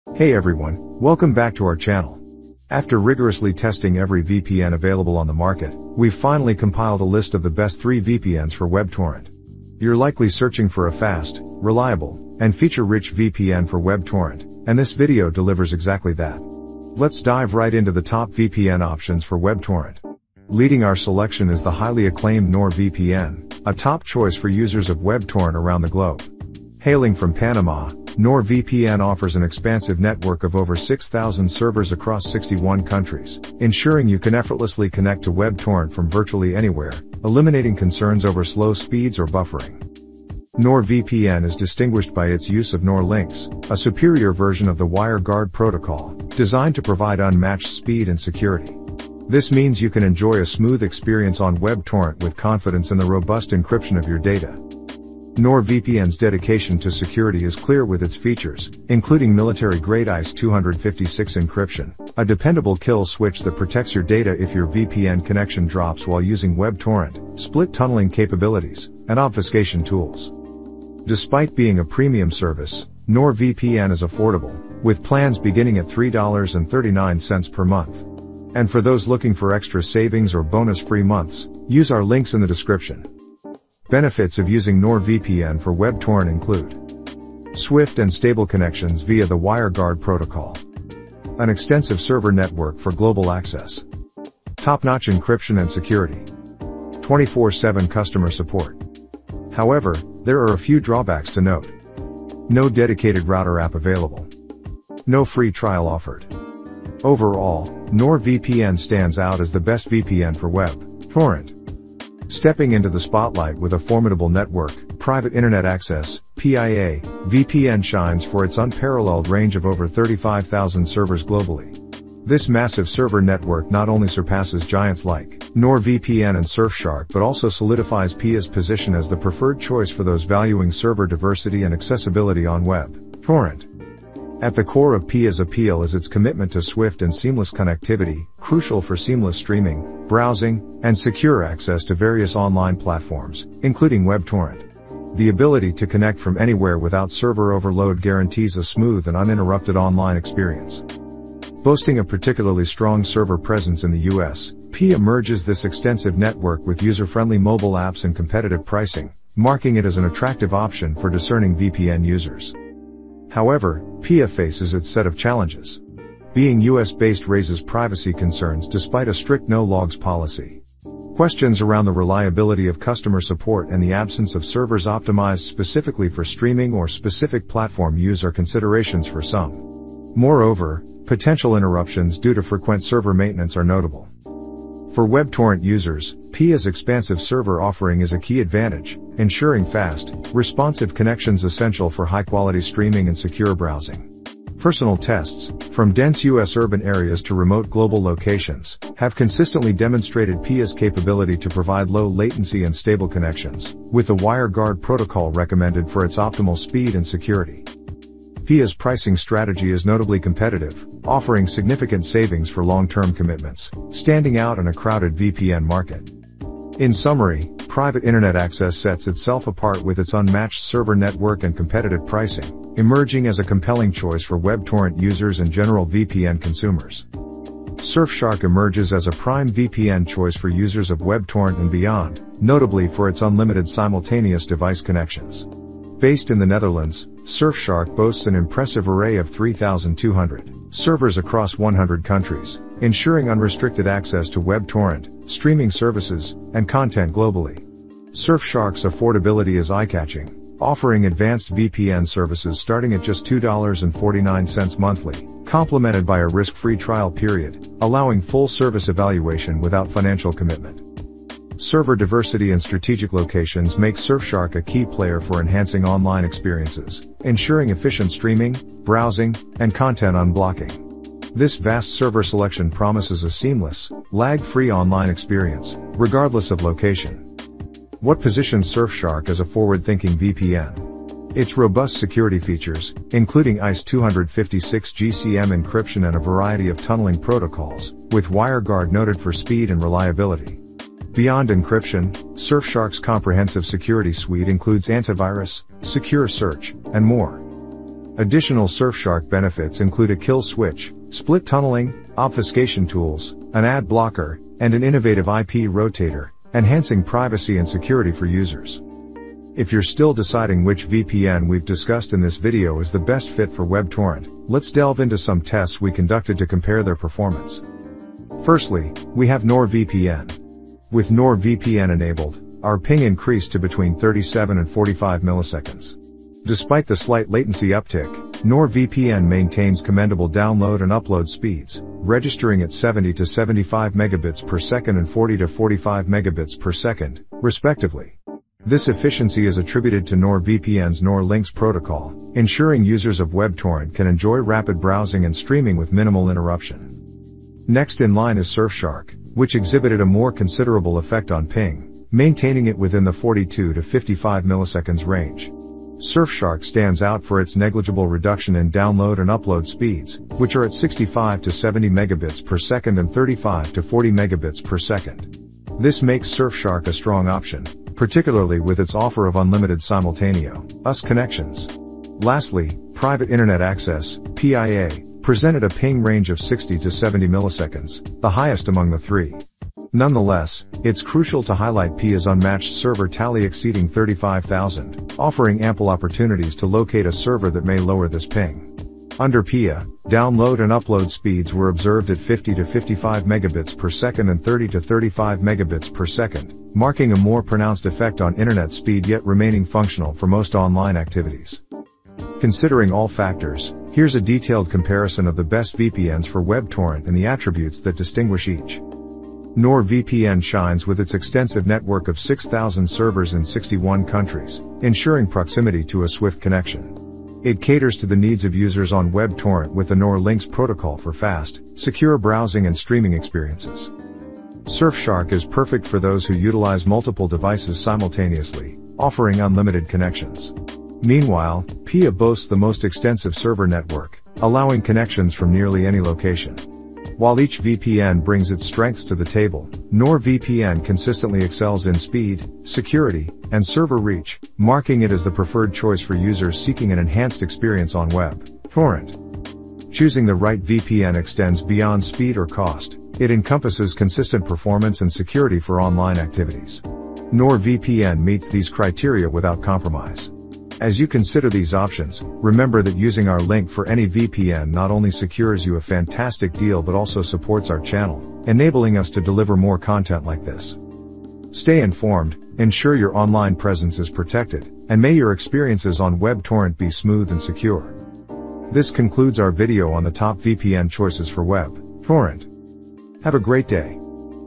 Best VPNs for WebTorrent - Top Budget Picks for 2024
In this video, I cover the top VPNs for WebTorrent and how to choose the best option for yourself based on your own requirements.

👉NordVPN is the best choice for WebTorrent if you take into consideration all aspects of a VPN.

Finding the best VPN for WebTorrent is crucial for ensuring your privacy and security online while also saving you money.
In this comprehensive guide, we'll explore the top three VPNs tailored specifically for WebTorrent, offering unbeatable value and peace of mind.

Leading the pack is NordVPN, widely acclaimed as the best VPN for WebTorrent and for good reason.

Next up is Private Internet Access (PIA) VPN, another top contender for the title of best VPN for WebTorrent. PIA VPN boasts an extensive server network of over 35,000 servers worldwide, ensuring optimal performance and security.

Rounding out our list is Surfshark, a budget-friendly VPN solution perfect for WebTorrent.